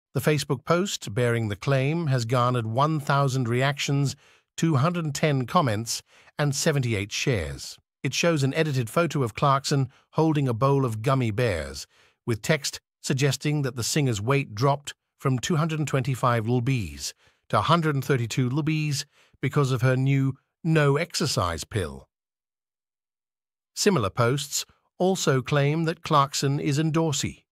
Kelly clarkson post bearing the claim has garnered 1,000 reactions, 210 comments, and 78 shares.
The Facebook post bearing the claim has garnered 1,000 reactions, 210 comments, and 78 shares.

It shows an edited photo of Clarkson holding a bowl of gummy bears, with text suggesting that the singer’s weight dropped from 225 lbs. to 132 lbs. because of her new “no exercise pill.”

Similar posts also claim that Clarkson is endorsing the product.